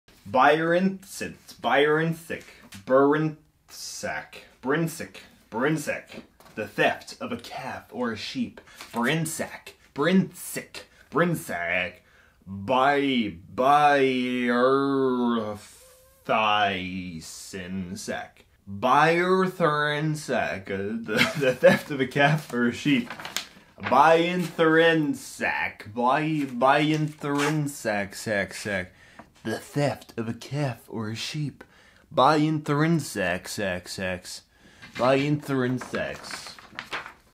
B: Byrthynsak: English Pronounce [Moribund Institute]
byrthynsak [See: Byrthynsak]
Byrthynsak - the theft of a calf or sheep
This video was edited by MoribundMurdoch